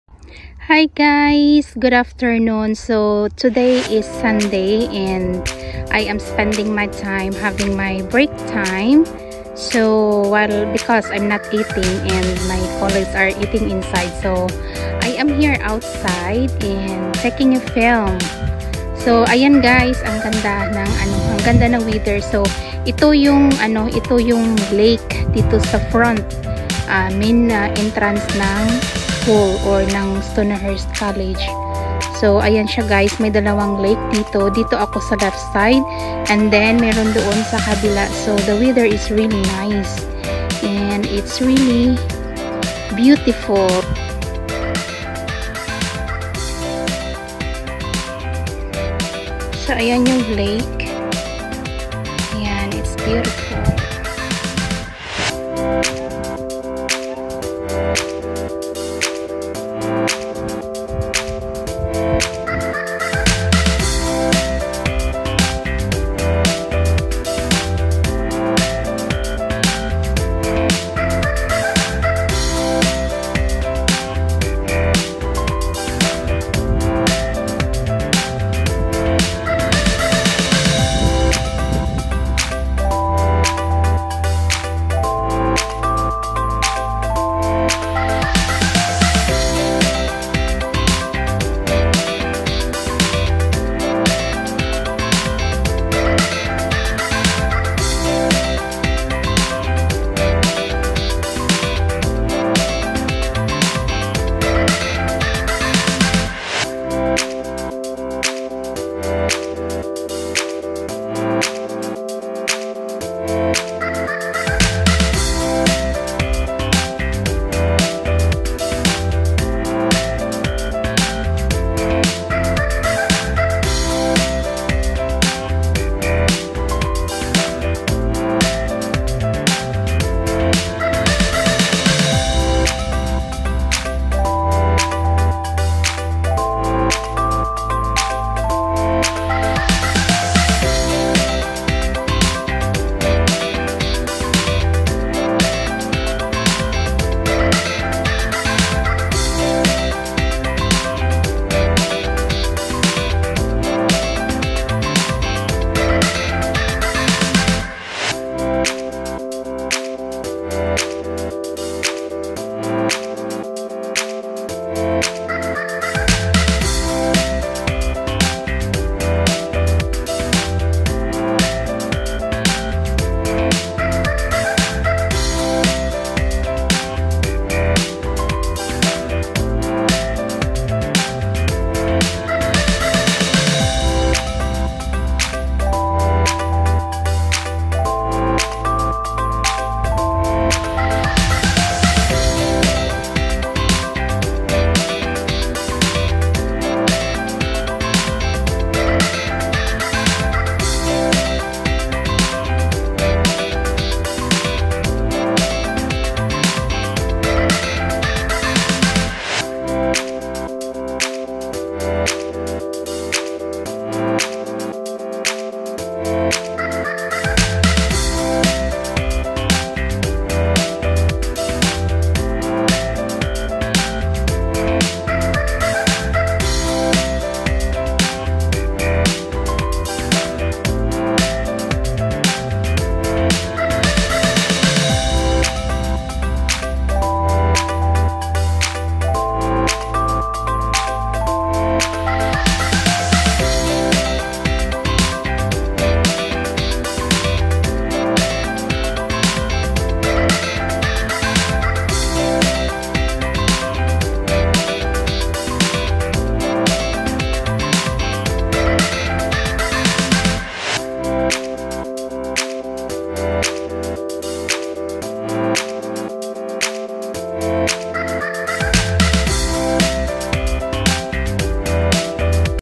Stonyhurst College, Ma Pa wow ka nalang sa Ganda ng View | Ghi Corr
I can't resist not to film this Stonyhurst college Main entrance Scenic view with garden, lakes and greenfield with  some animals. Also the weather is lovely as it's spring season.
Happy watching Guy's I hope you will like it.